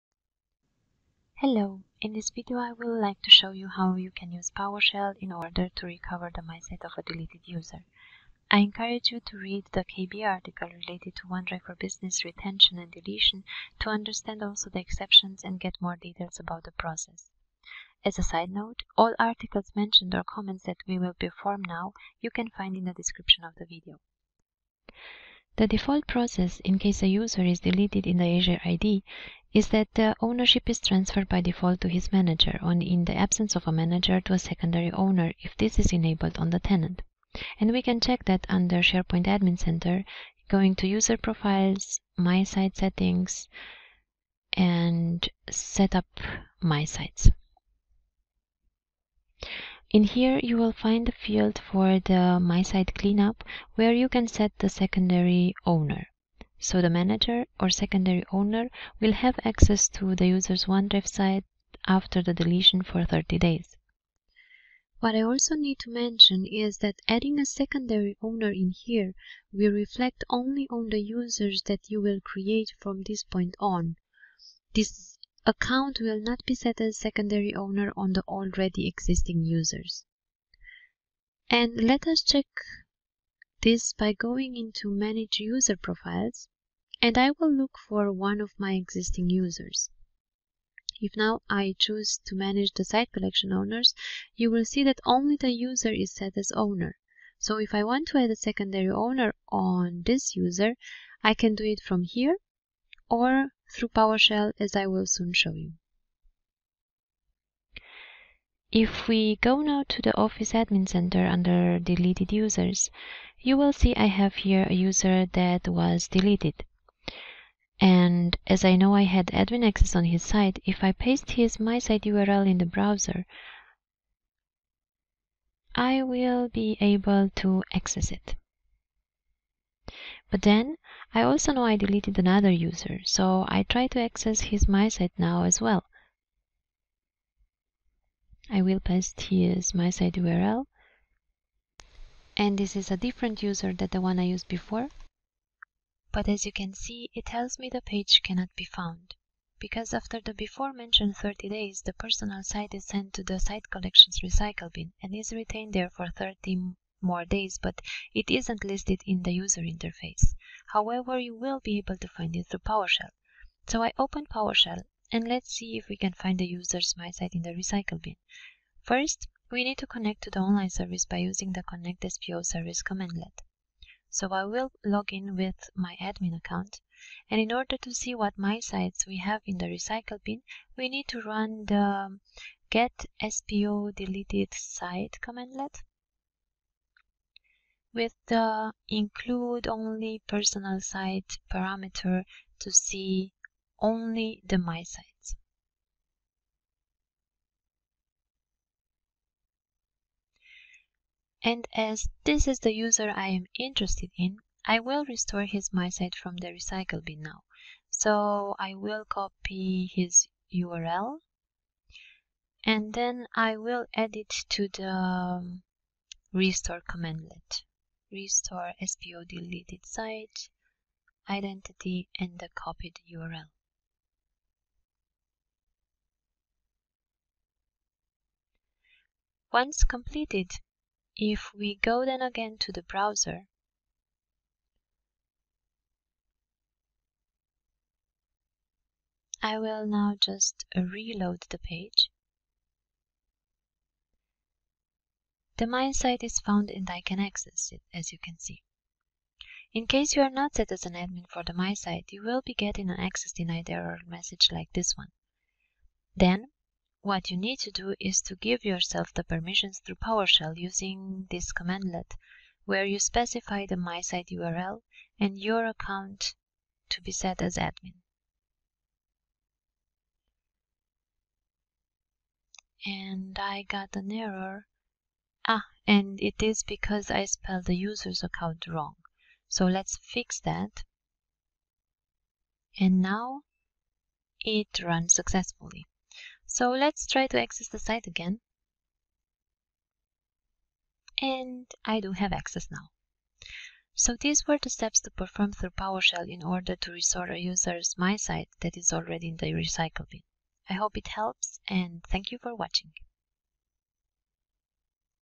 Short Video: OneDrive for Business - Recover deleted MySite (Powershell)
You want to restore a users MySite, but you cannot access it in the Portal anymore? You can check through Powershell if this is still in the recycle bin and this video will show you exactly how.

For more info to retention and deletion of MySites please read the following KB article

Video steps:

1. Download and Install Sharepoint Management Shell

Get-SPODeletedSite -IncludeOnlyPersonalSite | fl  - will return all MySites that are in the Recycle Bin of the sitecollection, but are not visible on the Portal

Restore-SPODeletedSite -Identity “URL of the personal site of the user till username”

3. Now go to the browser login to the with your office 365 credentials and paste the restore users OneDrive link

4. In case you are getting the error access denied, use the below command to set your admin account as secondary owner.

Set-SPOUser -Site “Url of the personal site till user name” -LoginName  “login ID of the user who want to be made the secondary admin” -IsSiteCollectionAdmin $true

Now, the restored OneDrive can be accessed by the users MySite URL.

Level 100 – Basic

Language: English

Technology: Sharepoint / onedrive

This video have been delivered by: Manuela Rusu-Hack